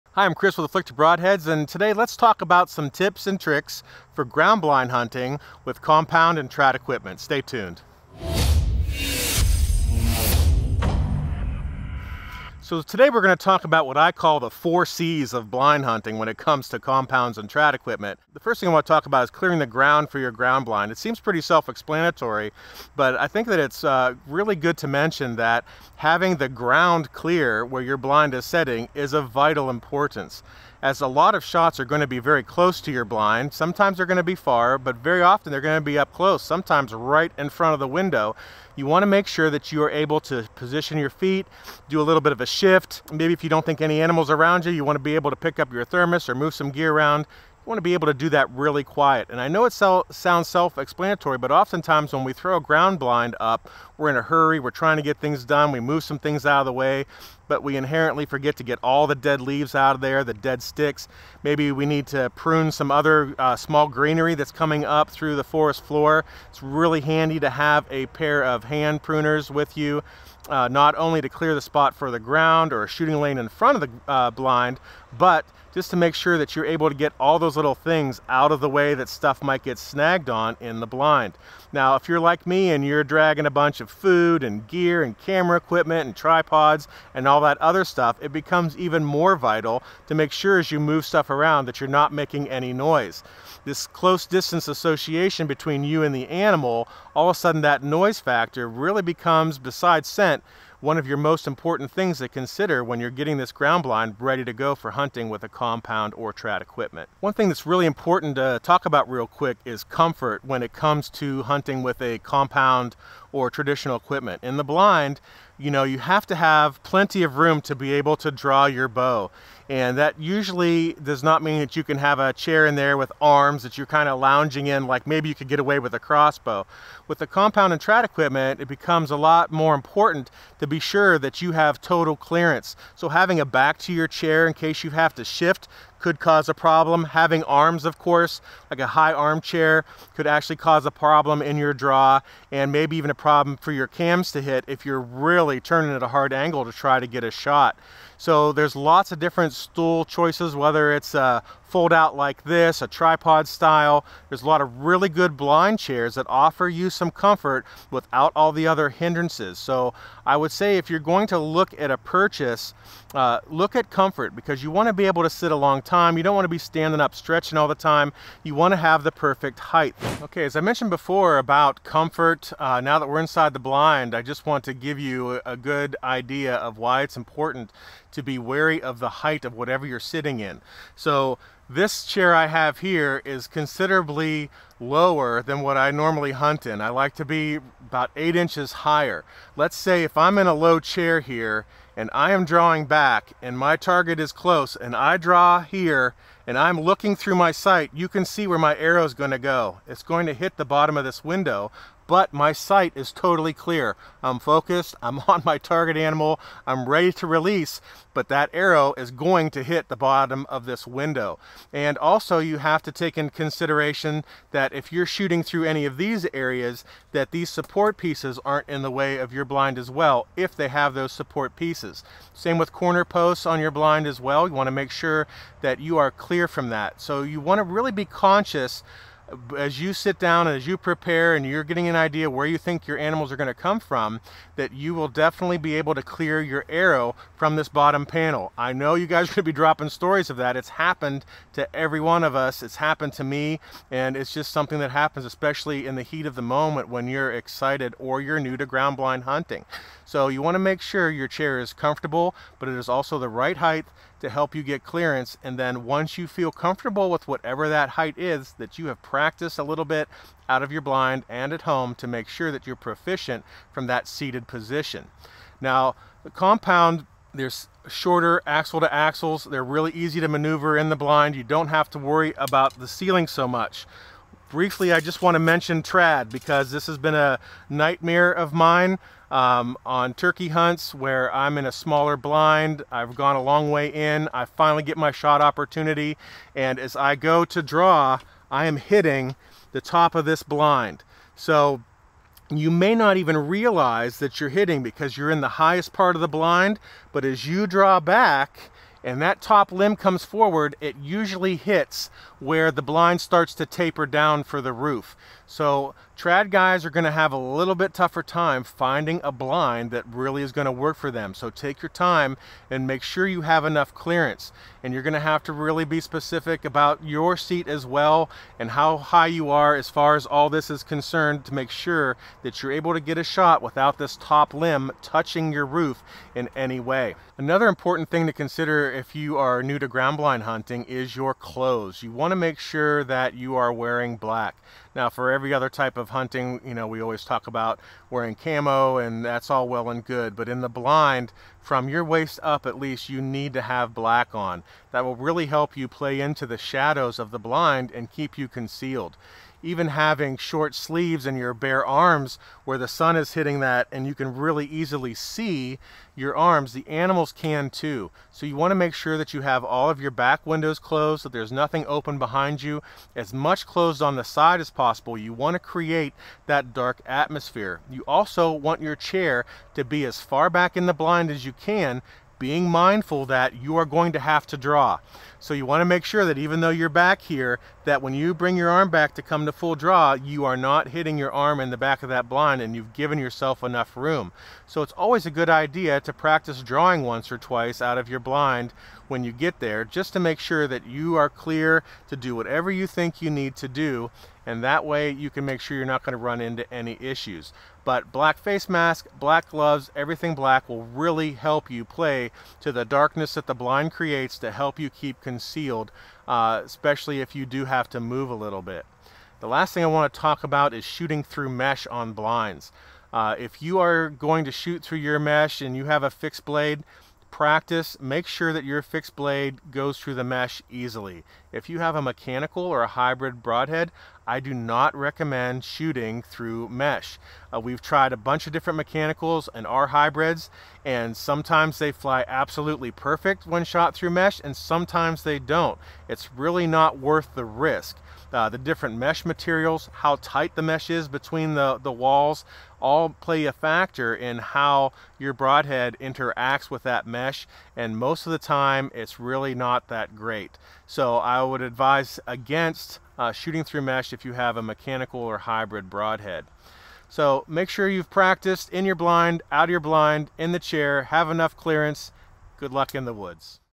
What YOU need to Know About Bowhunting From Ground blinds!
In this video, Chris Creed of Afflictor Broadheads gives us some tips about bowhunting from ground blinds using traditional bows and compounds.  There are a few things you need to consider when trying to use a ground blind to bow hunt and we're here to help.

The first tip is to clear the ground inside of the blind.  A lot of times when you are ground blind hunting, shot opportunities come at closer distances so even the smallest noise can be devastating to your hunt.  Keeping the ground clear allows you to make subtle movements without being detected.

The next thing to consider is comfort.  You want to hunt for long periods of time right?  Well being comfortable is a key factor to consider.  Along with comfort comes choosing the correct chair to hunt with from a ground blind.  If you are using a compound bow or traditional archery equipment, your chair choice is really important. You don't want anything with arms, or a back because if you have to shift to draw your bow, the arms may get in the way and the back of the chair is another thing that can make noise.

You also want to take note of the height of the chair because arrow clearance is one of the things that ruins a hunt quicker than anything from a ground blind.  Your site window doesn't always show the bottom of the blind, but your arrow could potentially be pointed directly at the blind.  So, get a chair that is going to allow you to clear the shooting window with your arrows.

The next thing to consider is your clothes. Typically when we talk about deer hunting we are talking about some type of camouflage.  In ground blinds you want to wear black everything.  At least from the waist up.  Blending in to your surroundings is the key.  Keep the windows closed as much as possible and the darkness will keep you hidden.

The last thing we want to touch on is shooting through the "shoot through" mesh.  We do not recommend shooting through this mesh with any mechanical or hybrid broadheads, and if you are shooting fixed blades we still recommend practicing with these heads through the mesh to see if your arrow is experiencing any odd flight.

Take these tips into consideration when ground blind hunting, and you will be good to go!  Good luck